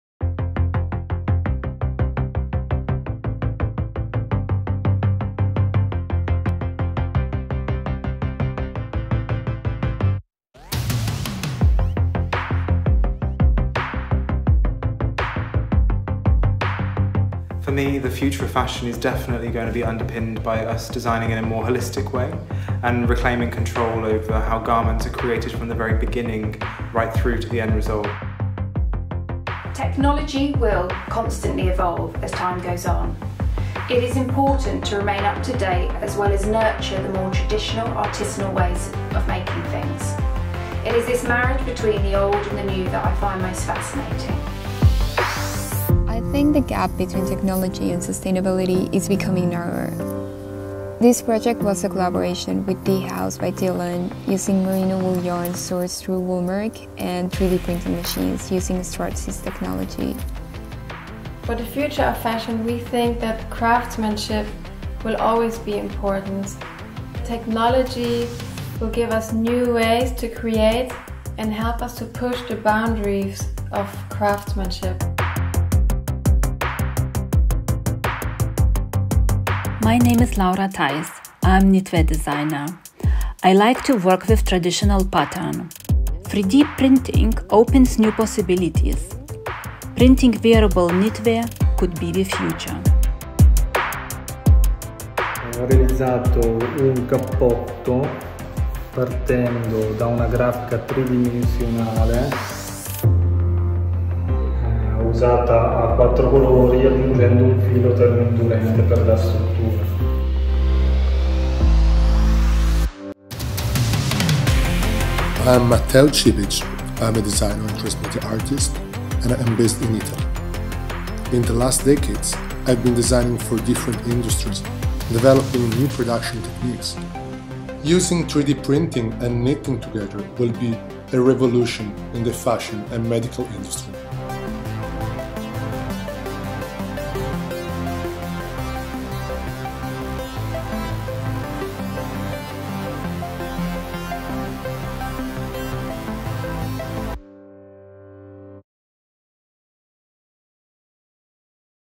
Knitting the future 3D printing meets Merino wool
The "Knitting the future 3D printing meets Merino wool" project, proposes a cutting- edge technological theme in the sector: 3D printng in combination with Merino wool Knitwear.
To show the versatility of possible applications, 3 international designers and 4 students from the Royal Collage of Art in London were involved, supported by as many Italian knitting and spinning mills. All the projects were made by 3D printing on Merino wool knitwear to create a unique combination.